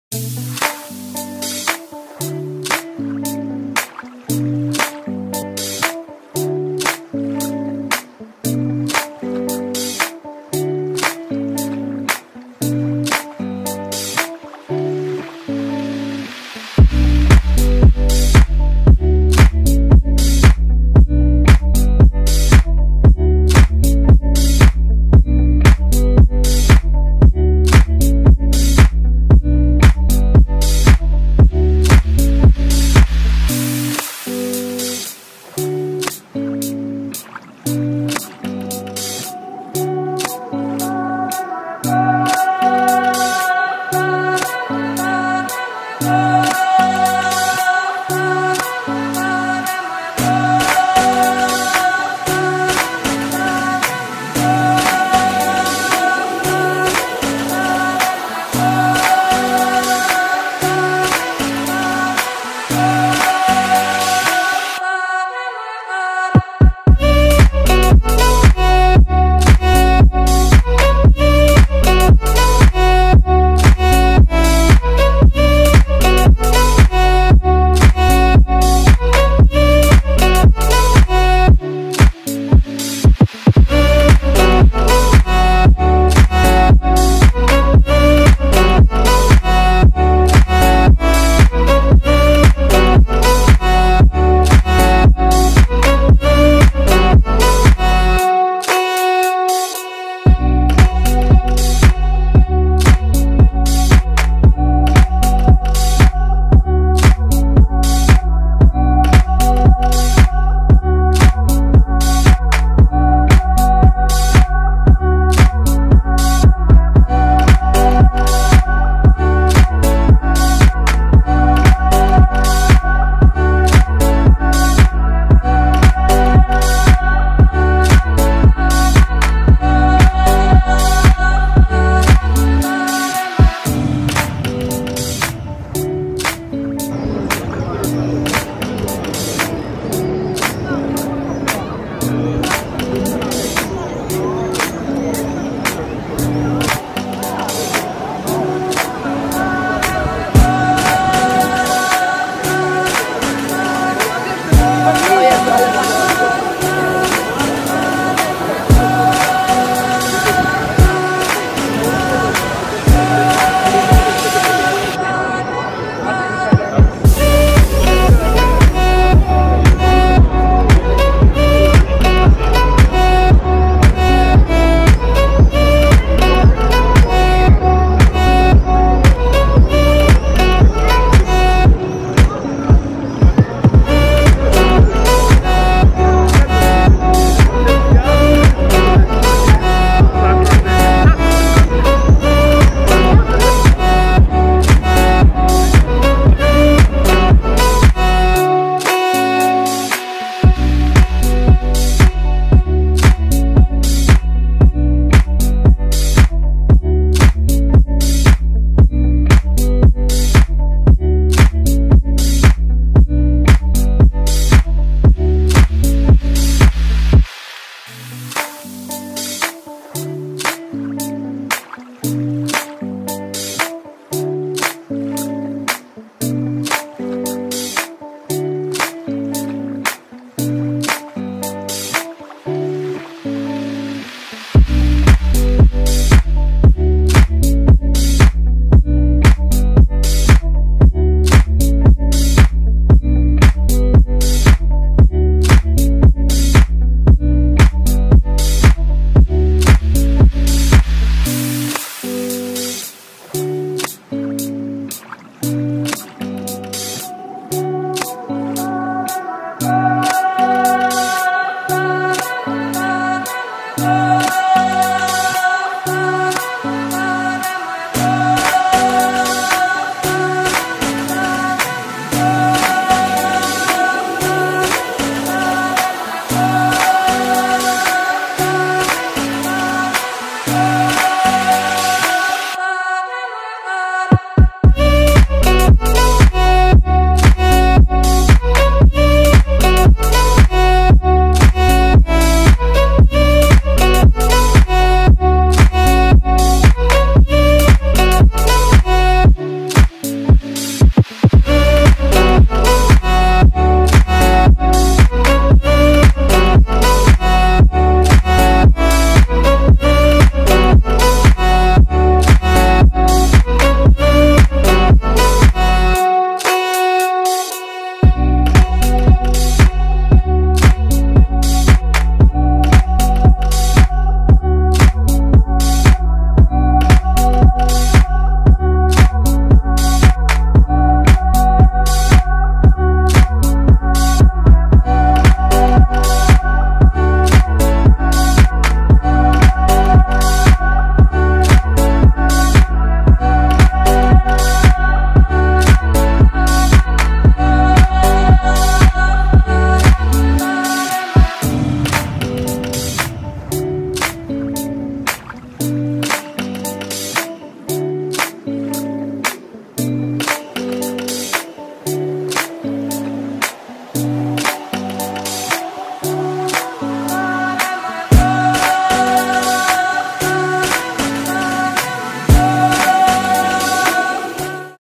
MY DIARIES | MUST VISIT IN KRAKOW POLLAND🇵🇱 | CHURCHES, CASTLE & SALT MINE| 2 Days 1 Night Trip
Hello everyone 😘